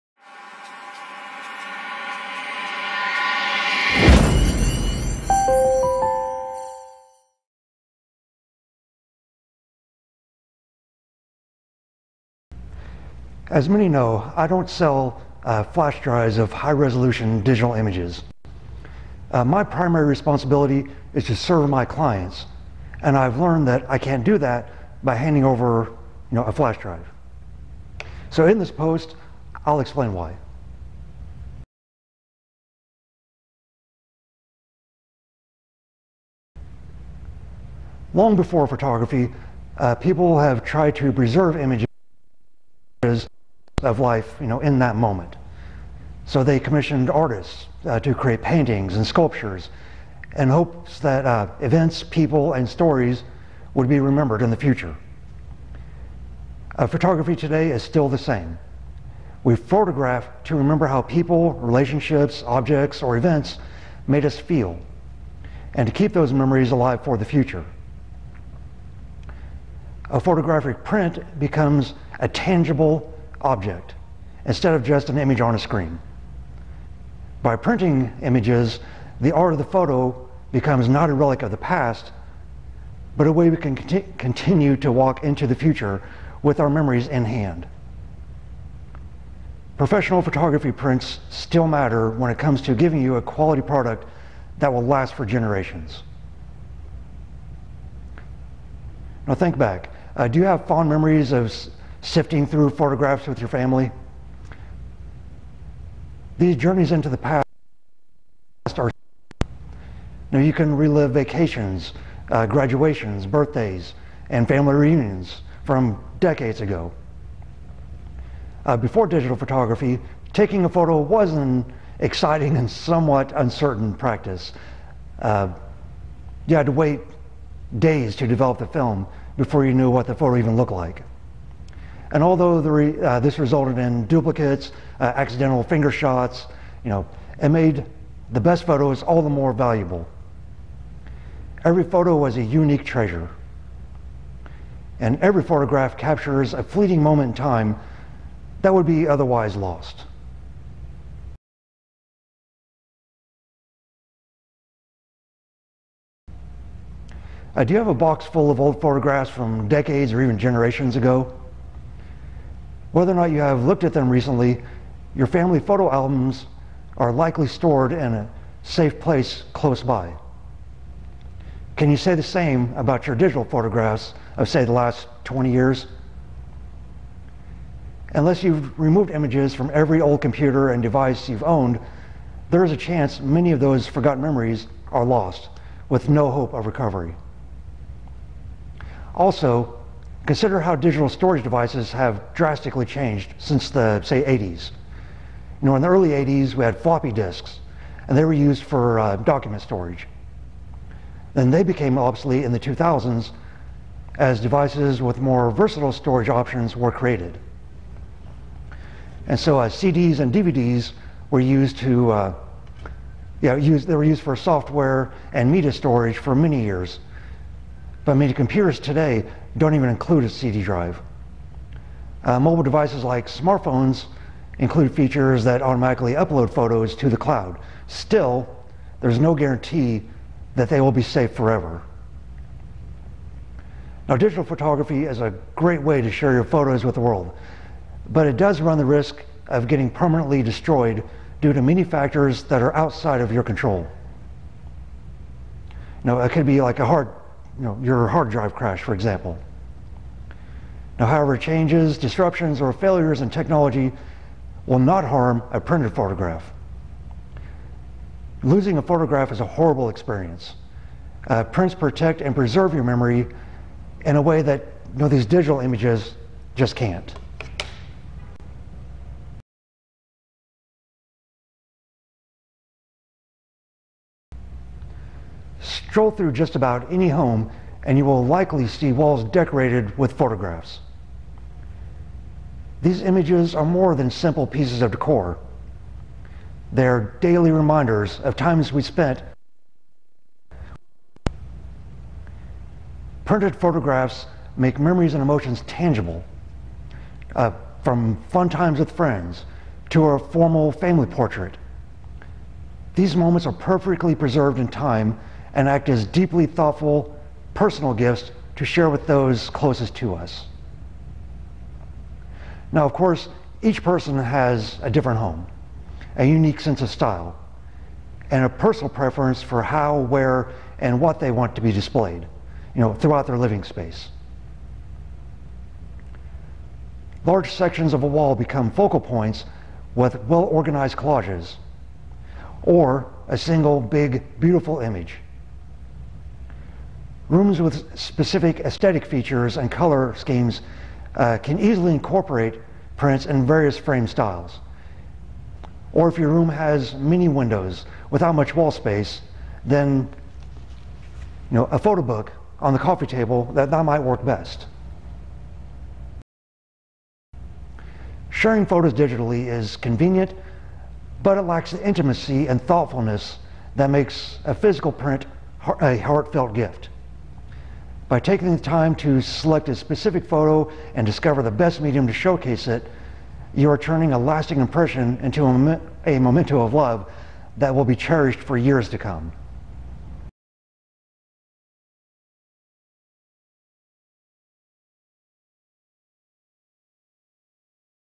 Why Prints Matter
Here, I explain just some of the reasons why physical prints are far superior to digital files